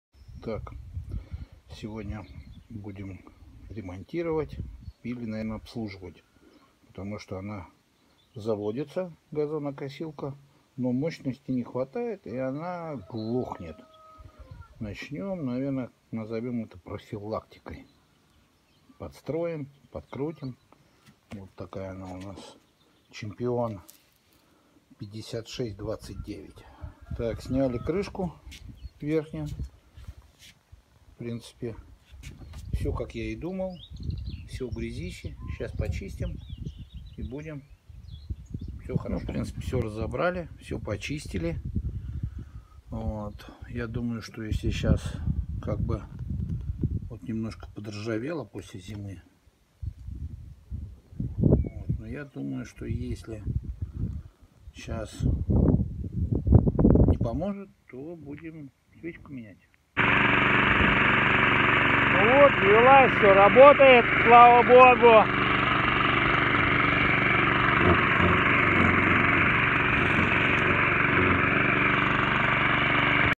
Глохнет газонокосилка Champion LMH 5629
Обслуживание газонокосилки Champion 5629